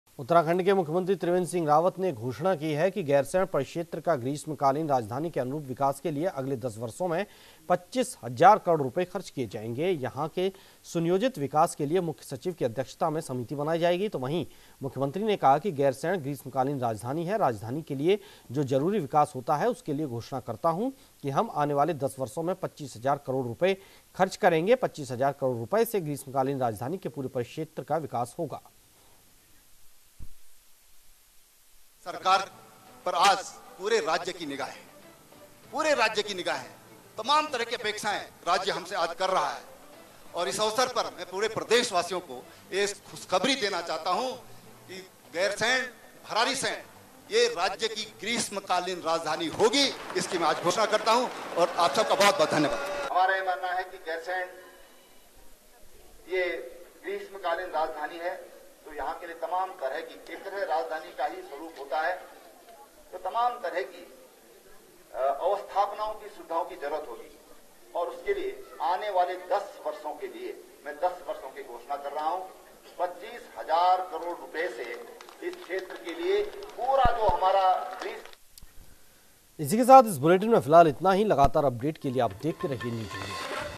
उत्तराखंड CM त्रिवेंद्र सिंह रावत ने की घोषणा अगले 10 वर्षो में 25 हजार करोड़ रूपए किये जाएंगे खर्च
News India 24x7 Live for Latest Updates & Breaking News of Politics, Sports, Cricket, IPL 2020, Bollywood, Health, Business, Lifestyle etc.